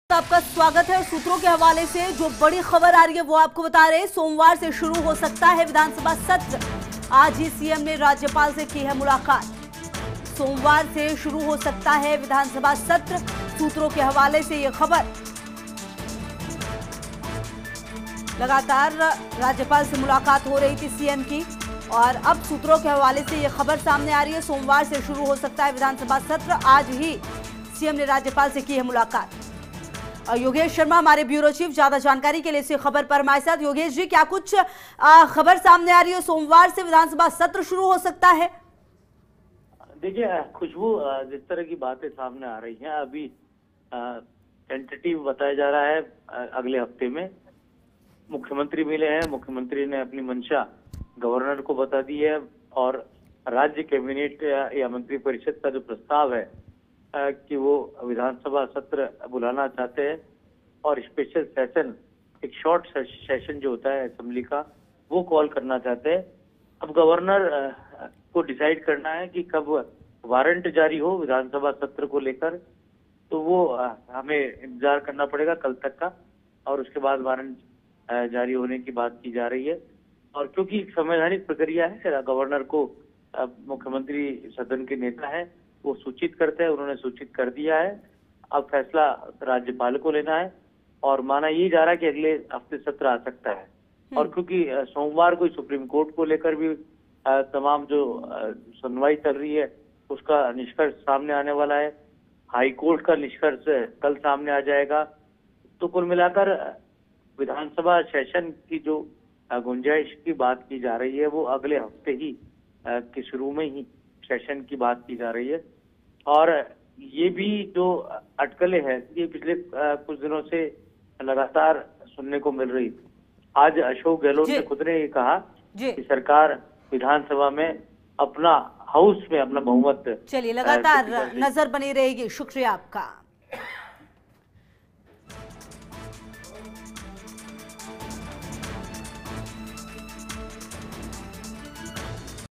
सोमवार से शुरू हो सकता है विधानसभा सत्र, CM Ashok Gehlot ने की राज्यपाल kalraj Mishra से मुलाकात
About this Video:
Assembly session may begin from Monday, CM Ashok Gehlot meets Governor kalraj Mishra
About Channel: